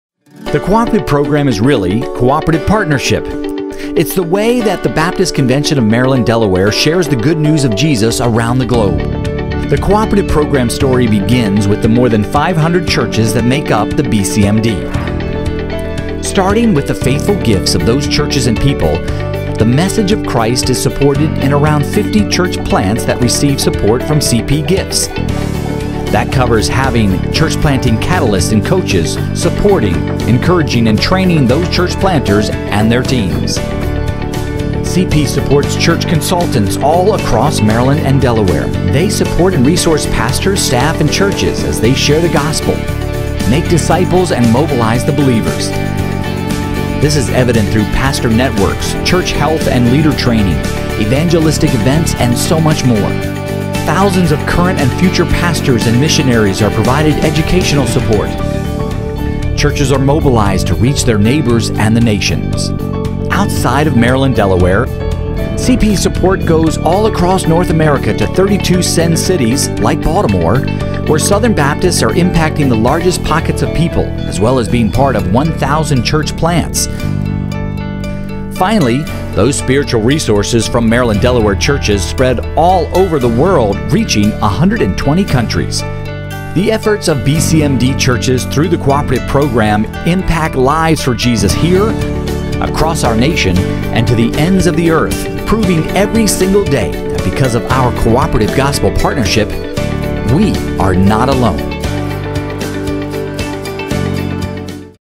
What is Cooperative Program?
Learn more about Cooperative Program giving and how it helps spread the love and message of Christ around the world.